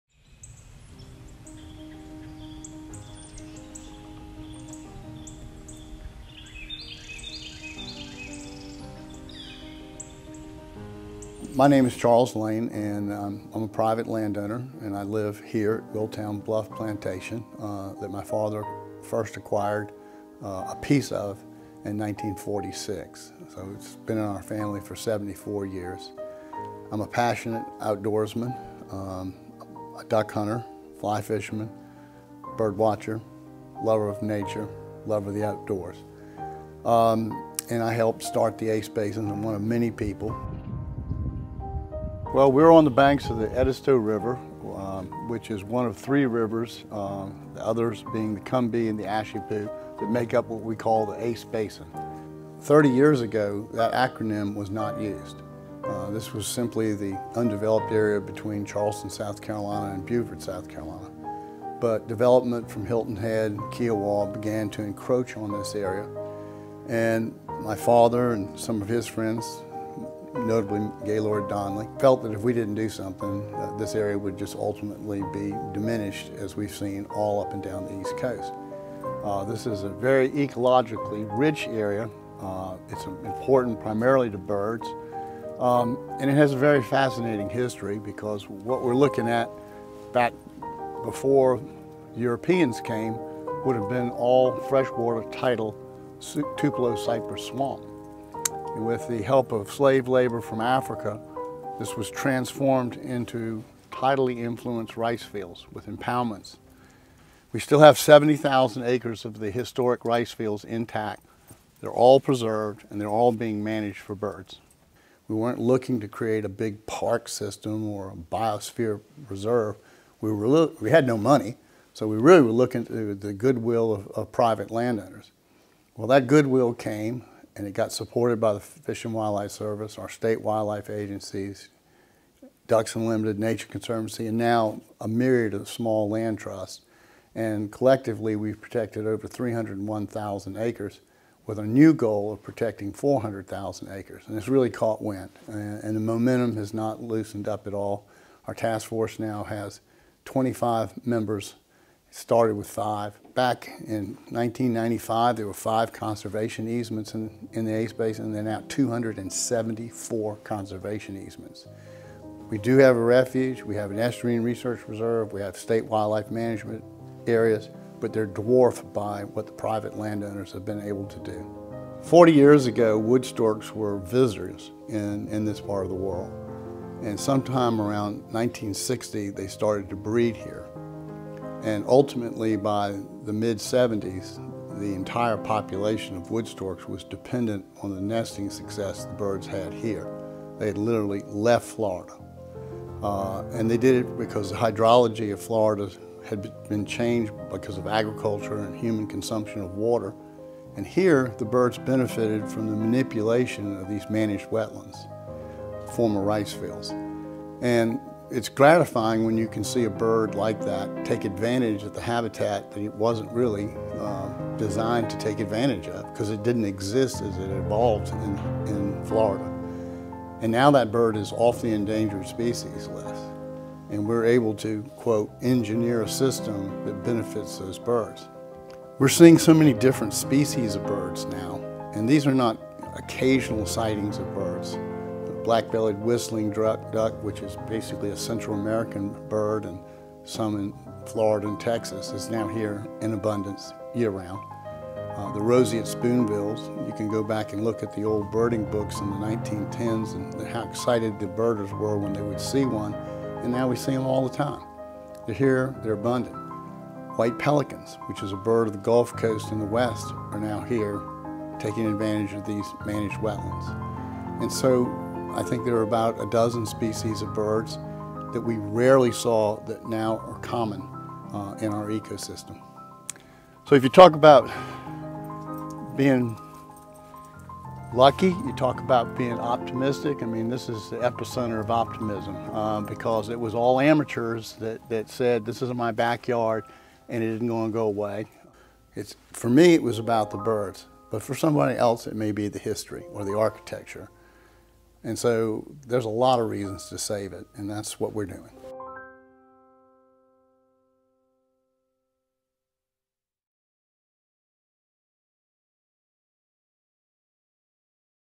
Bird Conservation at the ACE Basin
The ACE Basin – an acronym for the Ashepoo, Combahee, and Edisto Rivers – is an ecologically rich area near Charleston, South Carolina. Thanks to partnerships between private landowners, state wildlife agencies, and small land trusts, over 300,000 acres now comprise an area that is protected for bird and other wildlife habitat. Charles Lane, a founding member and former chair of the task force, talks about a few of the bird species whose populations have been bolstered due to preserved habitat at the ACE Basin.